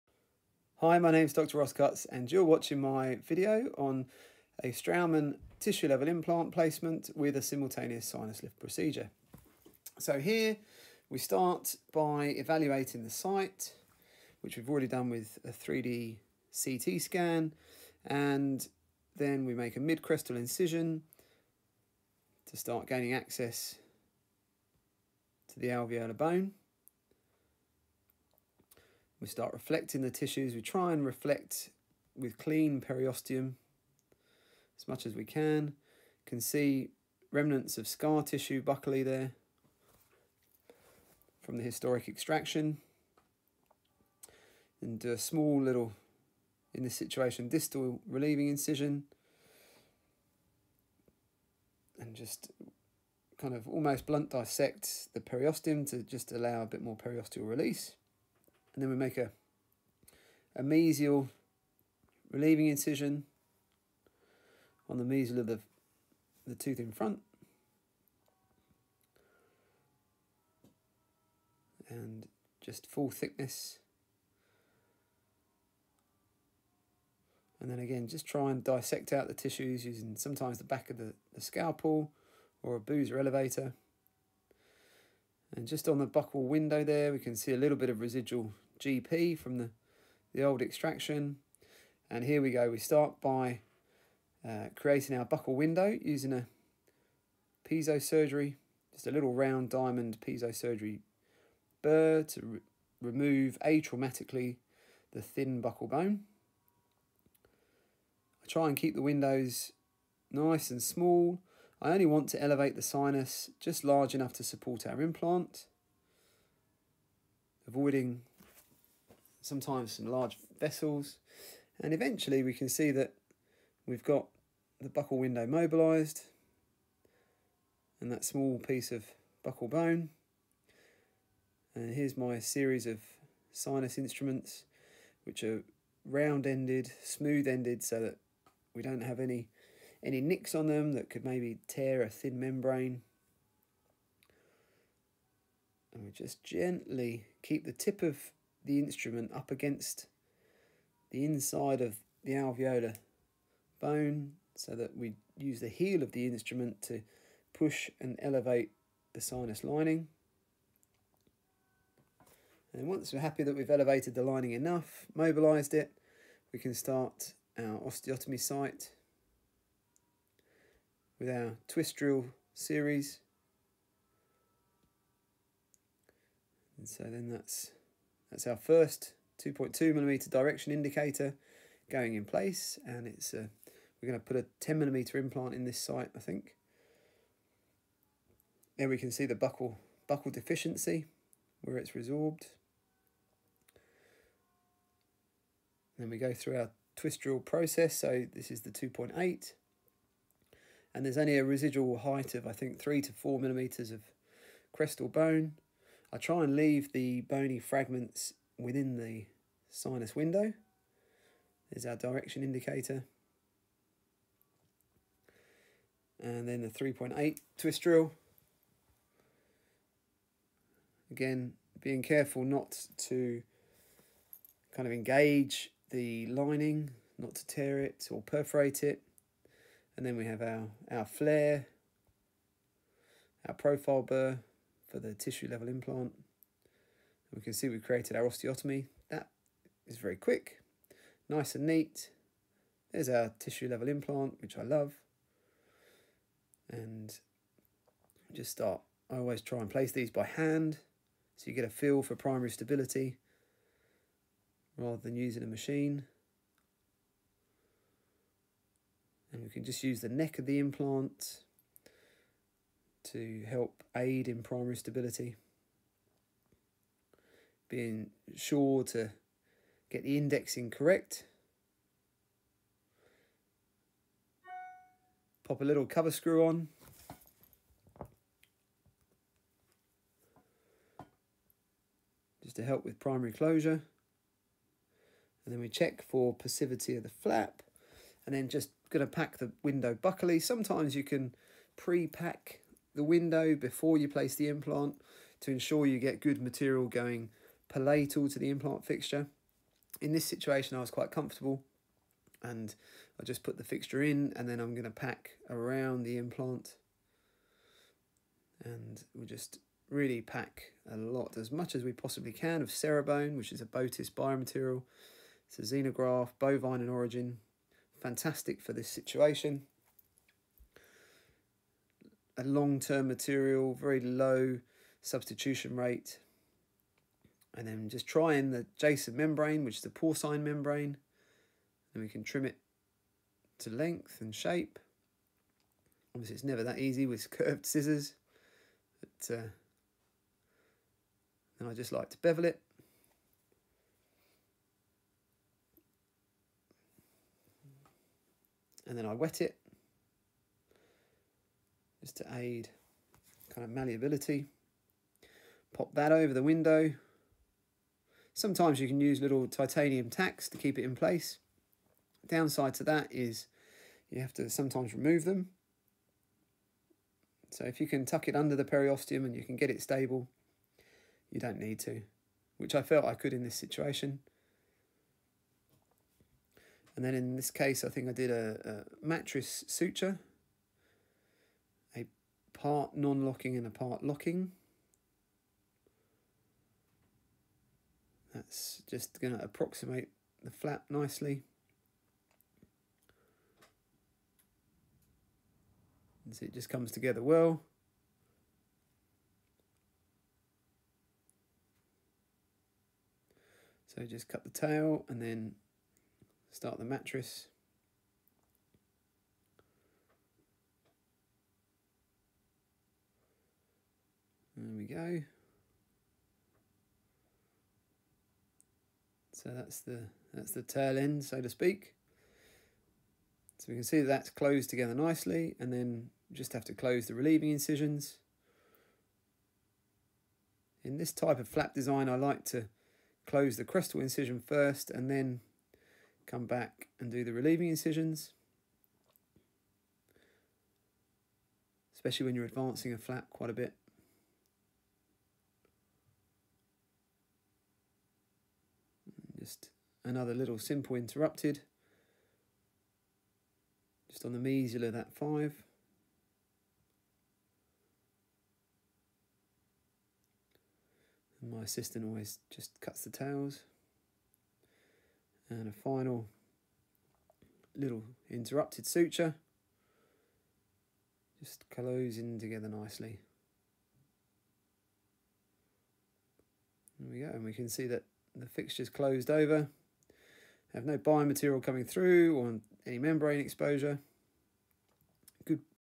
Dental Implant Placement and Simultaneous Lateral Window Sinus Lift
Placement of a Straumann Soft Tissue Level Dental Implant with a simultaneous full lateral window sinus lift procedure to create vertical height of bone